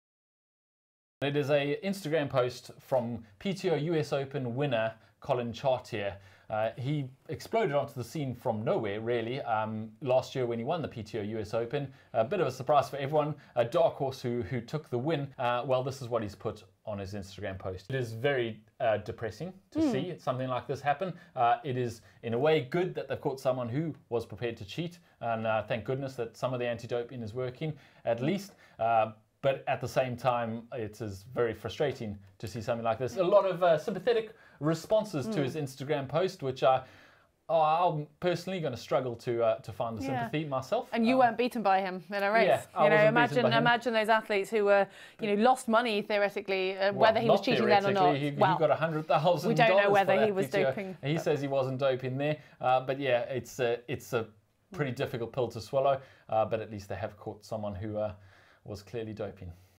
Pro Triathlete Collin Chartier Admits Taking EPO
We were mid-way through filming when we heard the news that Collin Chartier had admitted to testing positive for banned substance EPO. All of us at GTN are shocked and disappointed with the news that sends shockwaves through the sport that we love. Here is James & Heather's immediate response to the news breaking, we will be sharing our full, thought out response in an episode of the GTN Show that will be released tomorrow.

Useful Links
GTN Training Plans 👉 gcn.eu/gtntp